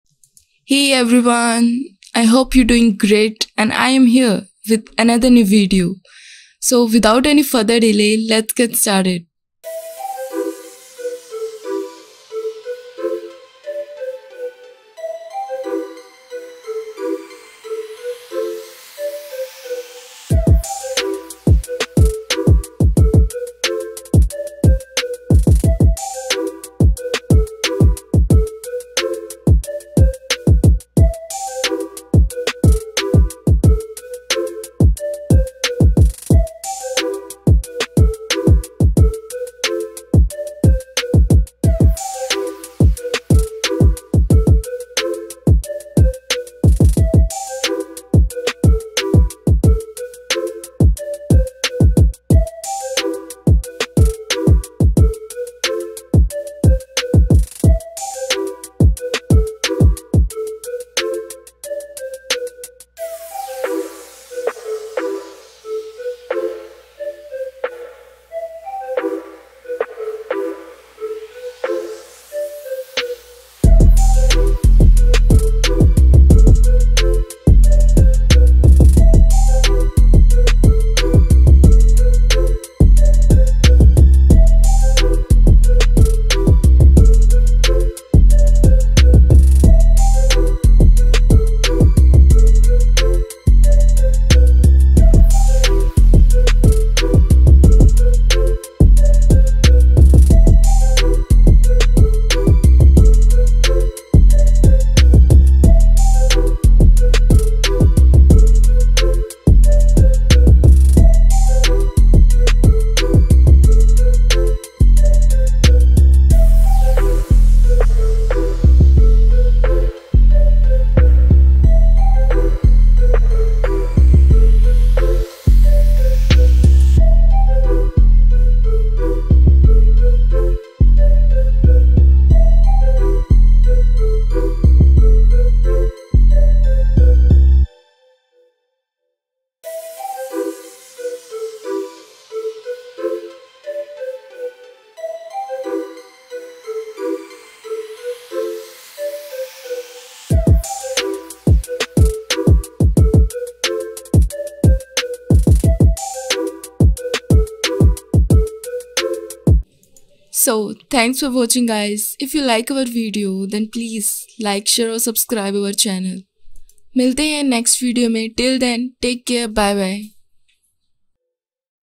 🔥Attitude quotes for girls🔥
👉Always act like you are wearing an invisible crown👑.”

👉“I may not be the girl that everyone wants, but at least I am not🙅‍♀️ the girl that everyone’s.  had.”❤️

👉His story is History, my story is Mystery.”😎

👉 “I love the confidence that makeup gives me.”

Girls quotes , quotes  , attitude Quotes
👉Life changing quotes
👉Go after dreams not people